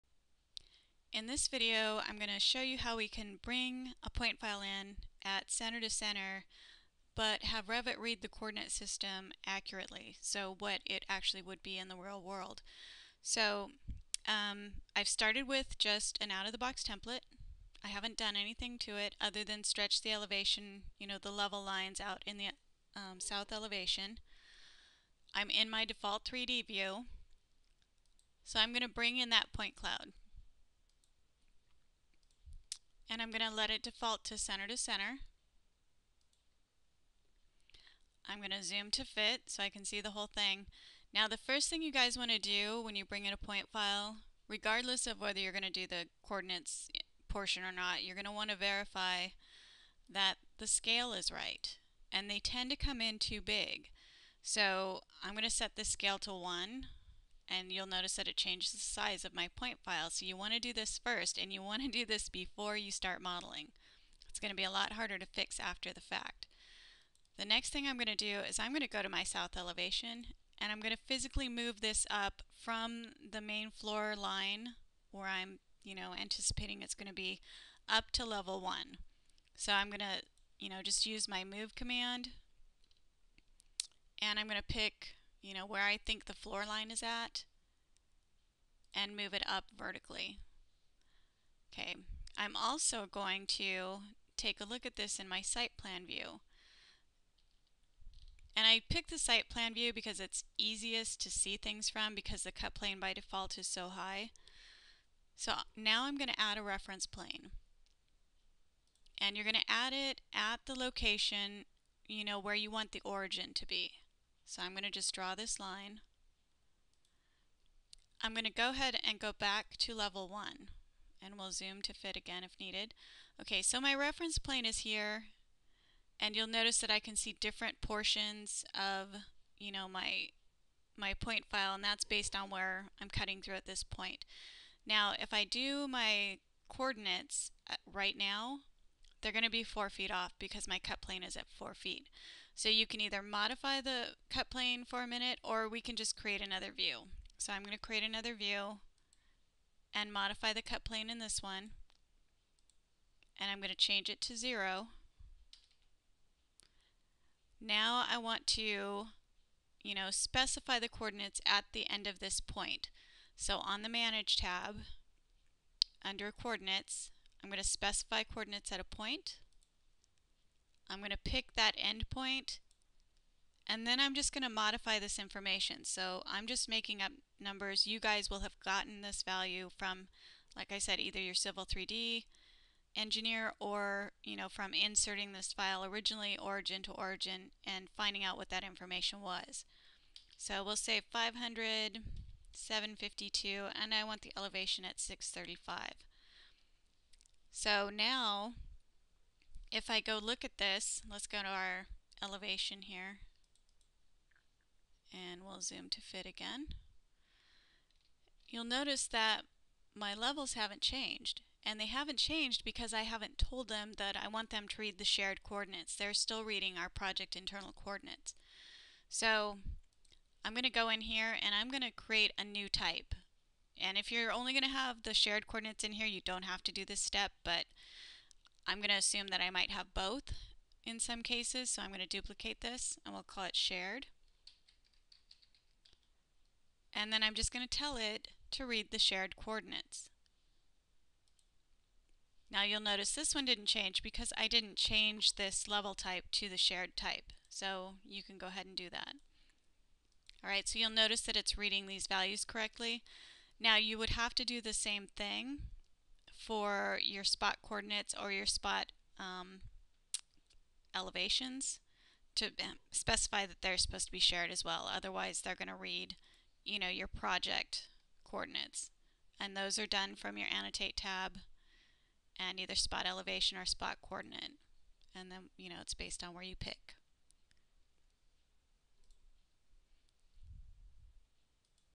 Point File Coordinates
How to bring in a point cloud center to center, but specify the correct coordinates.